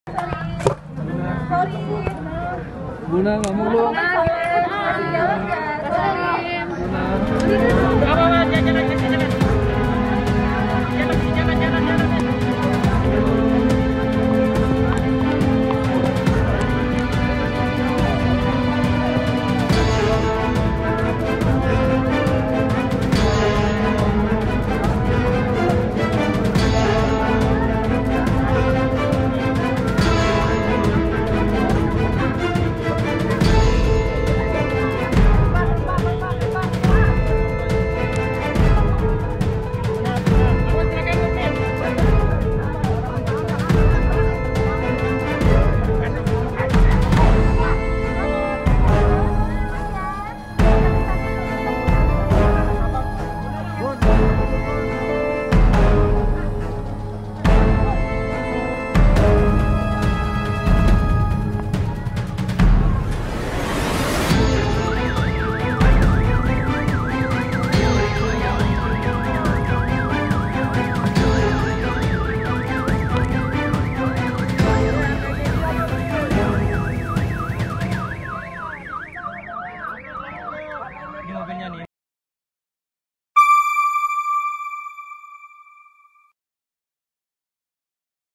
Rachel Vennya Ogah Kasih Penjelasan Pascapemeriksaan Sebagai Tersangka
Selebgram Rachel Vennya selesai jalani pemeriksaan di Polda Metro Jaya pada Senin (8/11), sebagai tersangka atas kasus pelanggaran karanita kesehatan.

Rachel Vennya bersama Salim Nauderer yang keluar dari Gedung Ditreskrimum Polda Metro Jaya pukul 14.20 WIB tidak menjawab dan diam tidak memberi keterangan saat dihadang wartawan.

Reporter: Dody Chandra
Penulis naskah: Eko Agustio Vi